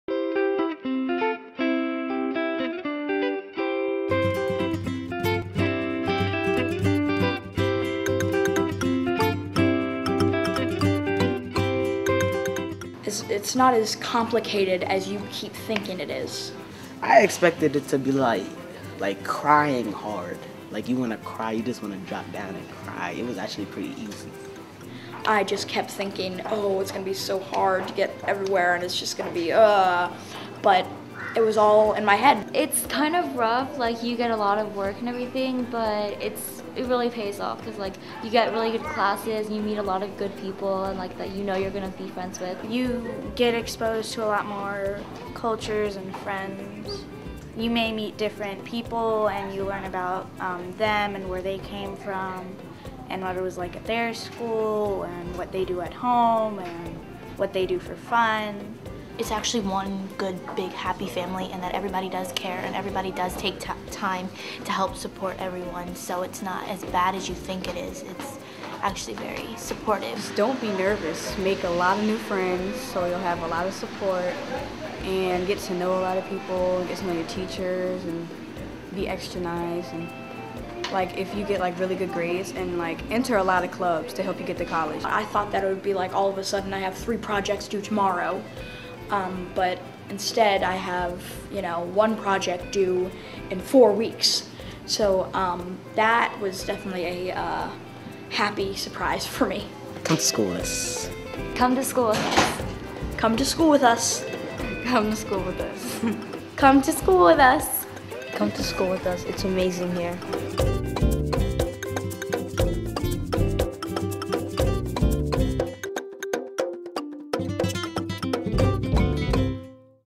COME TO MIDDLE SCHOOL WITH US!
STUDENTS FROM GEORGE WASHINGTON MIDDLE SCHOOL AND FRANCIS C HAMMOND MIDDLE SCHOOL RECALL THEIR EXPERIENCES OF FIRST COMING TO MIDDLE SCHOOL